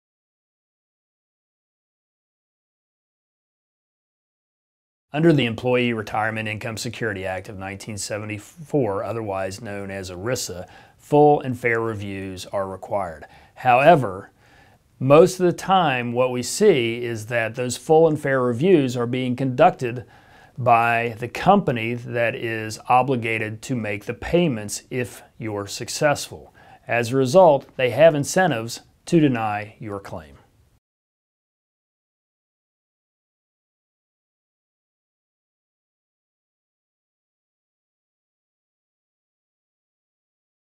Doesn't ERISA require my benefit plan conduct a full and fair review of my claim?
Doesn't ERISA require my benefit plan conduct a full and fair review of my claim? | McKinney, TX Office: 2530 W. White Avenue, Suite 200, McKinney, TX 75071 | Huntington, WV Office: 923 Third Avenue, Huntington WV 25701.

Under the Employee Retirement Income Security Act of 1974, otherwise known as ERISA, full and fair reviews are required. However, most of the time what we see is that those full and fair reviews are being conducted by the company that is obligated to make the payments if you're successful. As a result, they have incentives to deny your claim.